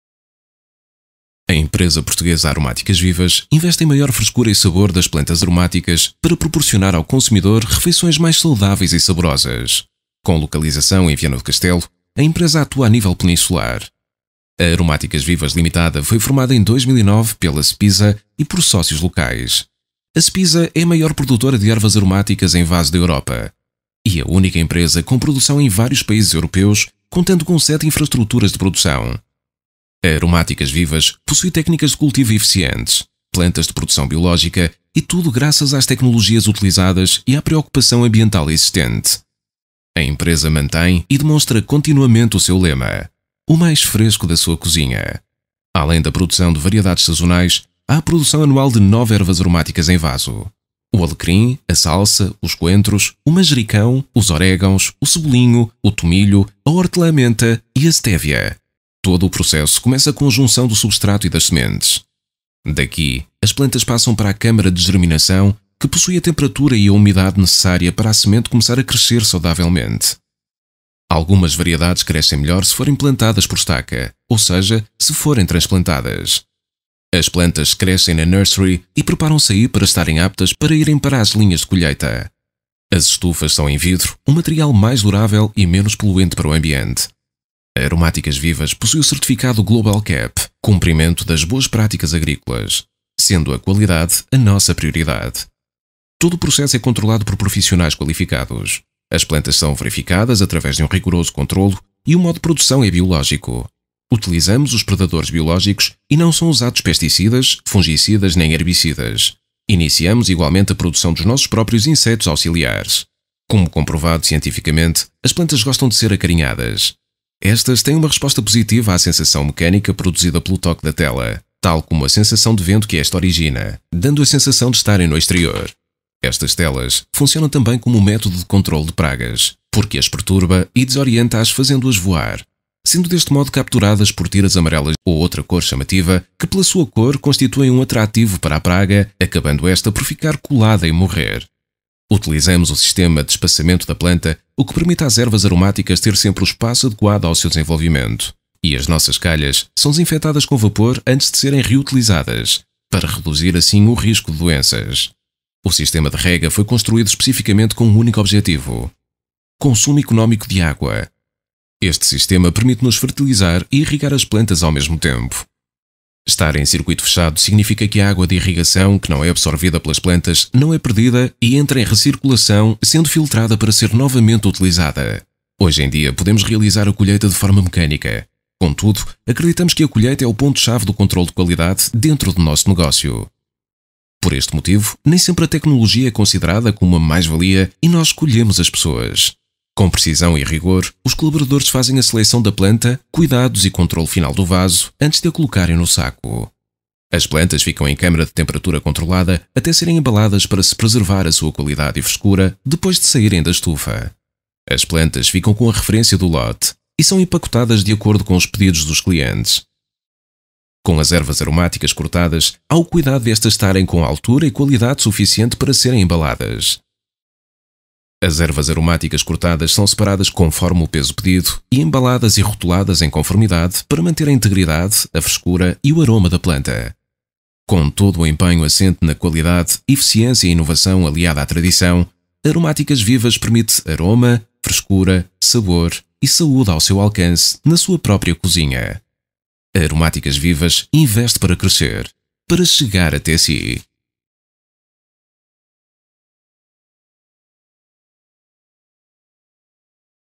Conheça a Aromáticas Vivas...
Neste vídeo pode conhecer as nossas instalações e algumas das nossas técnicas de produção.